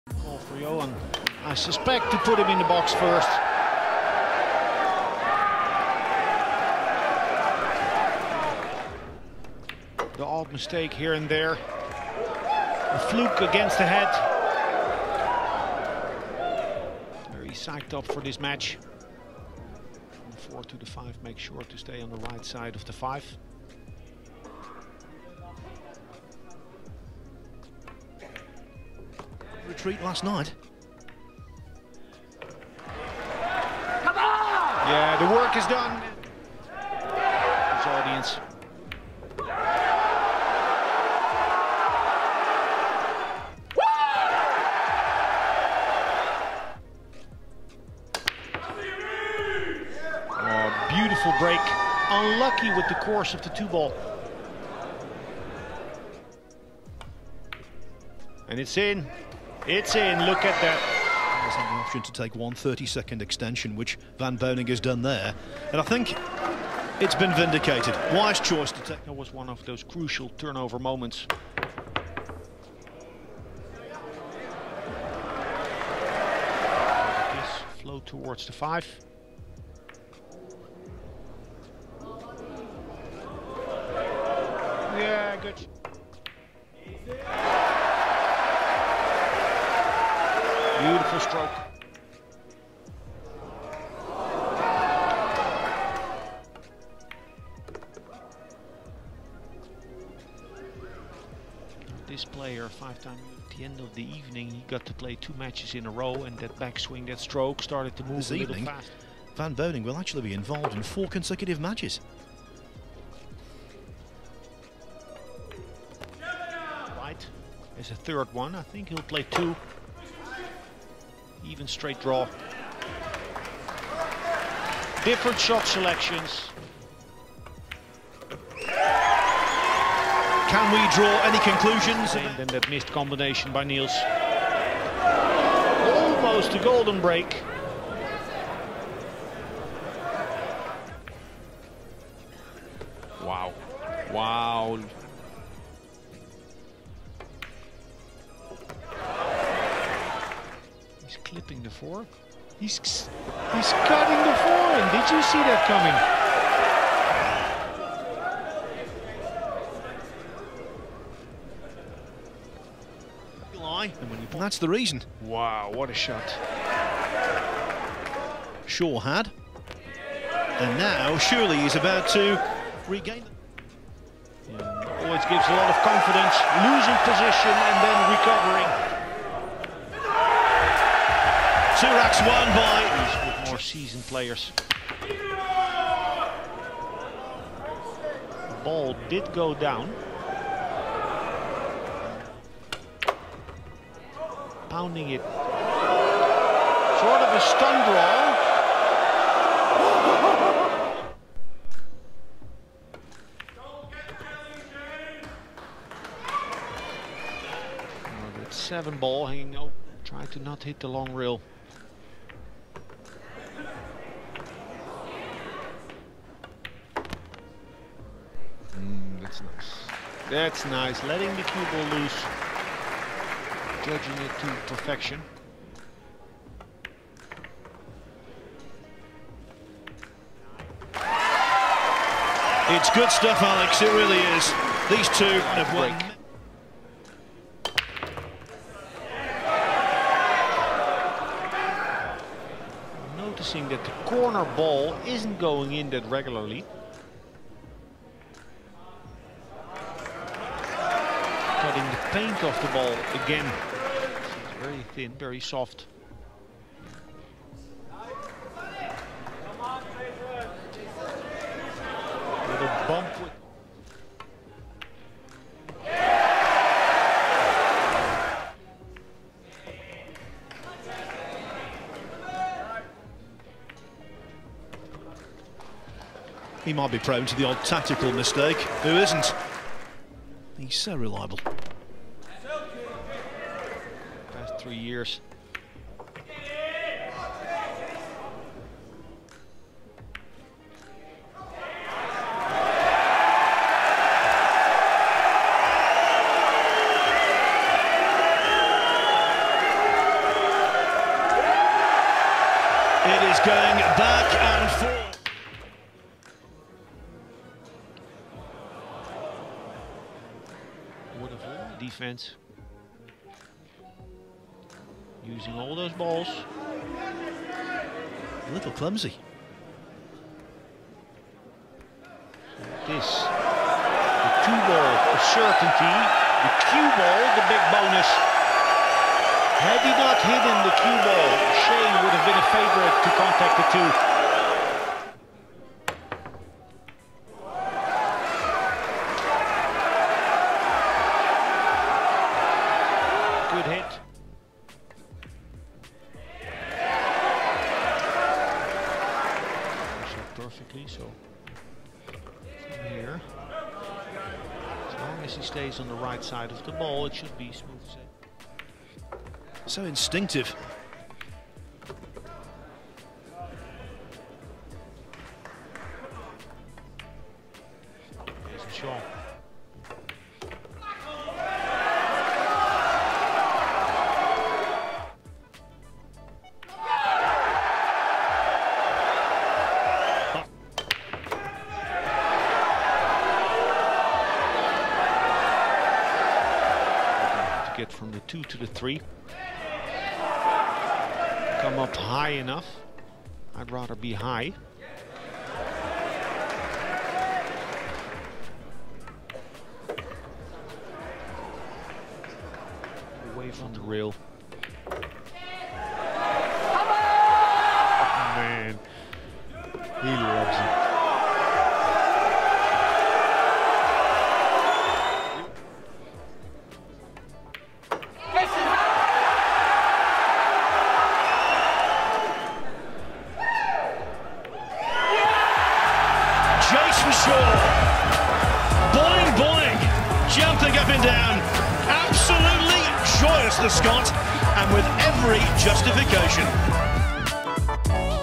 Speed Run: Jayson Shaw vs. Shane Van Boening Mosconi Cup 2018 - INFAMOUS CHAIR MATCH
With the 2020 Mosconi Cup just days away, it's time to relive one of the most electric matches in Mosconi Cup history. Jayson Shaw vs. Shane Van Boening. Two of the game's most dominant players face off for the first game of the 2nd day in 2018. Jayson is fresh from a defeat at the hands of Shane the night before and is ready to exact his revenge. Stay until the end because this match features the infamous chair stand that caused Tyler Styer to come through swinging and potentially caused a turning of the tide.